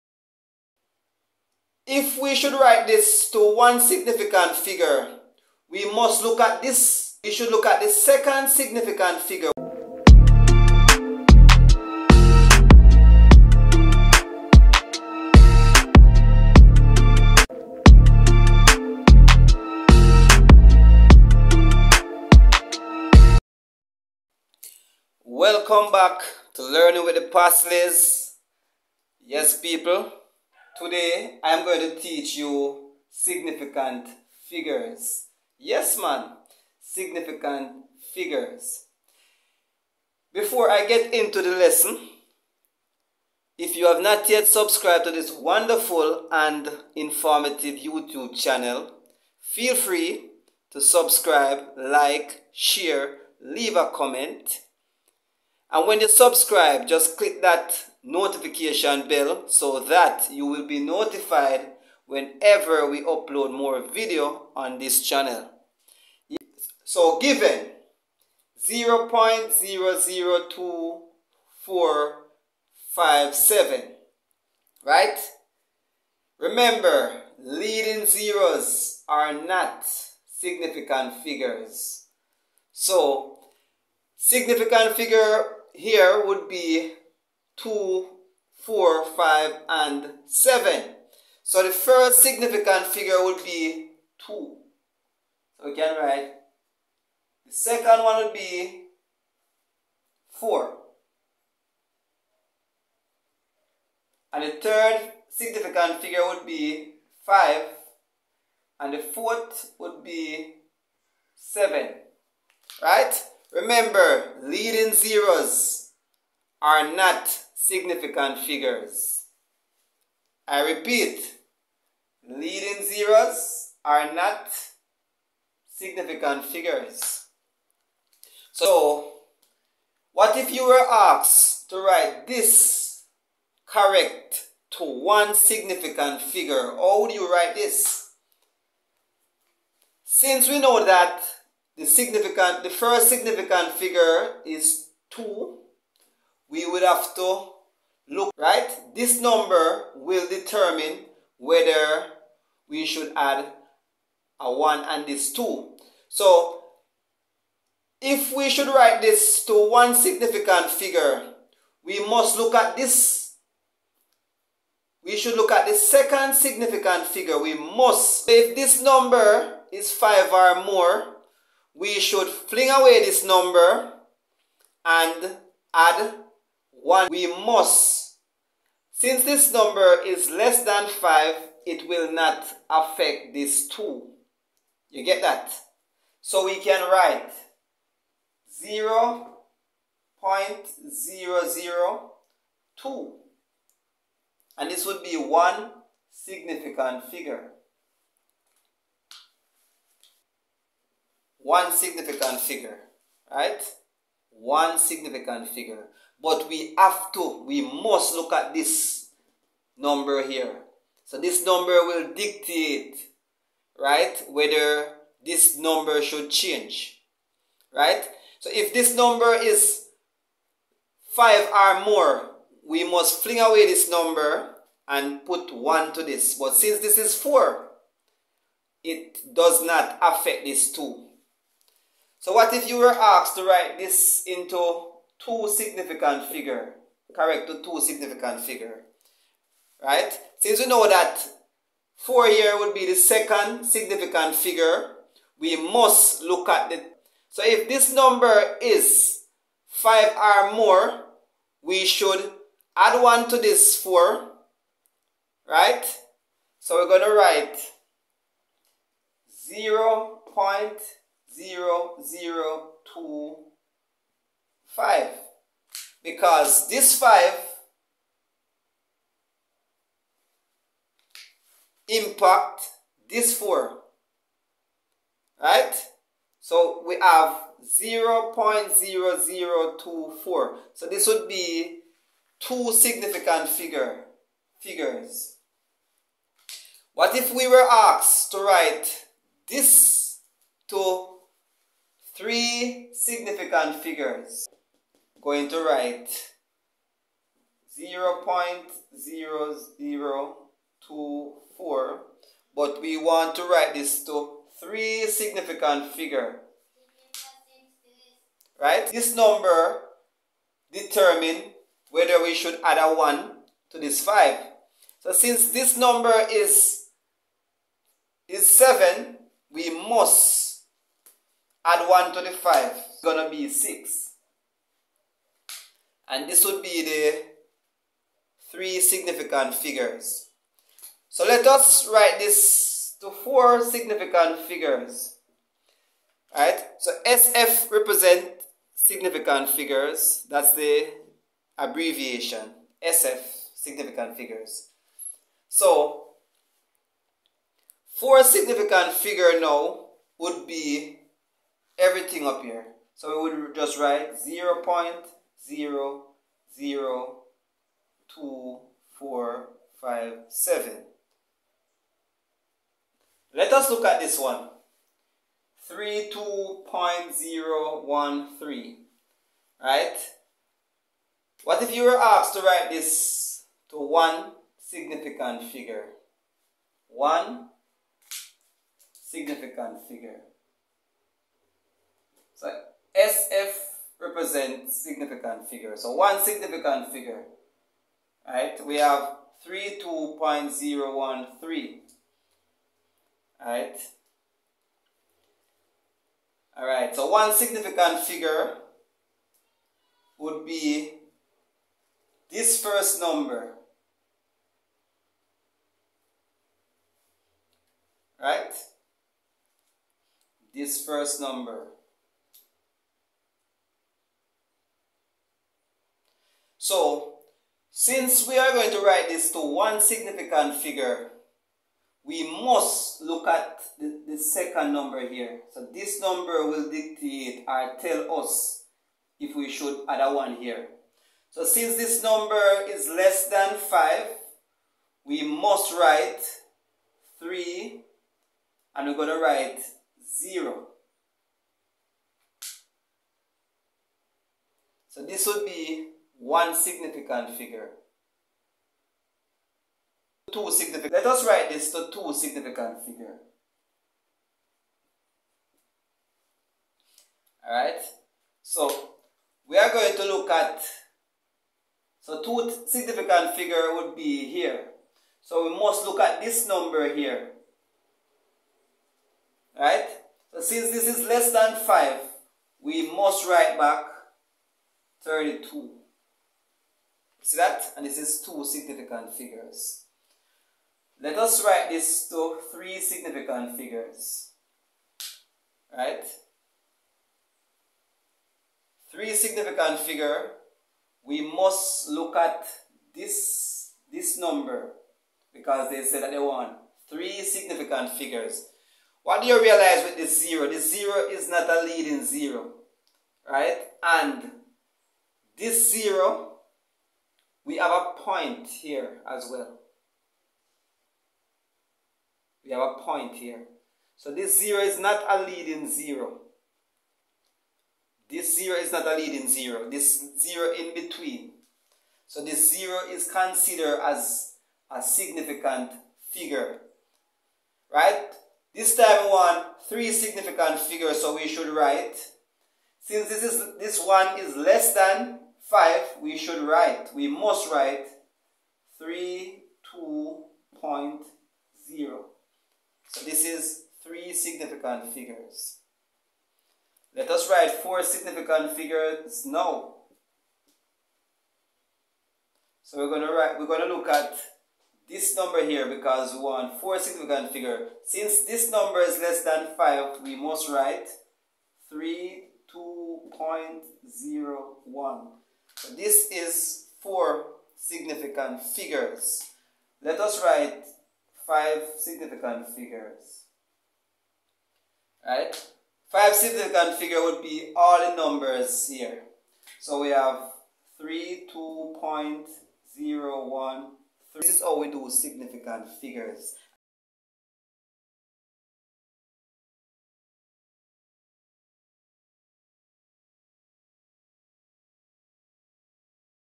SIGNIFICANTLY! made easy Significant Figures-CXC Mathematics |Learning with the Passleys
In this video, I  explained how to write numbers into Significant Figures.